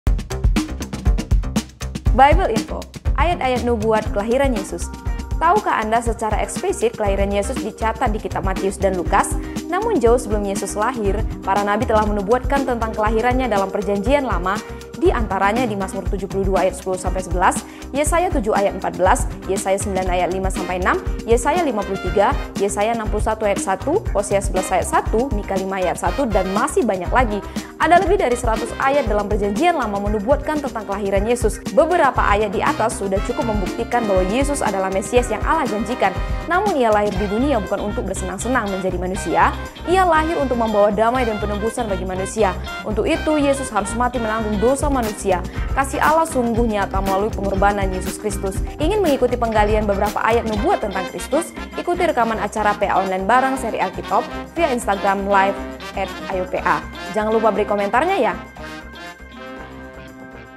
SABDA5 Desember 1: Bible -- Ayat-Ayat Nubuat Kelahiran Yesus
SABDA 5 - Desember 1: Bible Info
Ayat-Ayat Nubuat Kelahiran Yesus

Tahukah Anda secara eksplisit kelahiran Yesus dicatat di kitab Matius dan Lukas? Namun, jauh sebelum Yesus lahir, para nabi telah menubuatkan tentang kelahiran-Nya dalam Perjanjian Lama, di antaranya di Mazmur 72:10-11; Yesaya 7:14; Yesaya 9:5-6; Yesaya 53; Yesaya 61:1; Hosea 11:1; Mikha 5:1; dan masih banyak lagi.

Ada lebih dari 100 ayat dalam Perjanjian Lama menubuatkan tentang kelahiran Yesus. Beberapa ayat di atas sudah cukup membuktikan bahwa Yesus adalah Mesias yang Allah janjikan. Namun, Ia lahir di dunia bukan untuk bersenang-senang menjadi manusia, Ia lahir untuk membawa damai dan penebusan bagi manusia. Untuk itu, Yesus harus mati menanggung dosa manusia. Kasih Allah sungguh nyata melalui pengorbanan Yesus Kristus.

Ingin mengikuti penggalian beberapa ayat nubuat tentang Kristus? Ikuti rekaman acara PA Online bareng seri Alki-TOP via Instagram Live @Ayo.PA. Jangan lupa beri komentarnya ya.